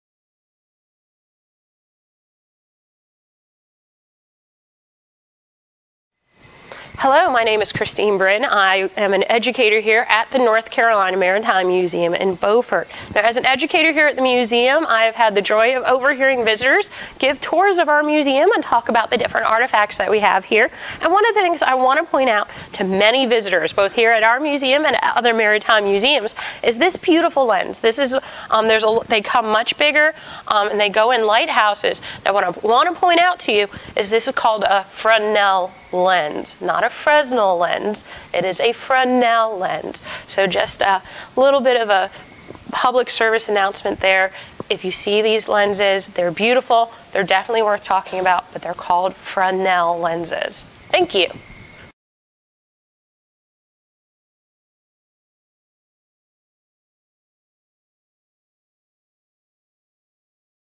Fresnel lens: how to pronounce it
Join Museum Educator Christine Brin to learn how to pronounce Fresnel lens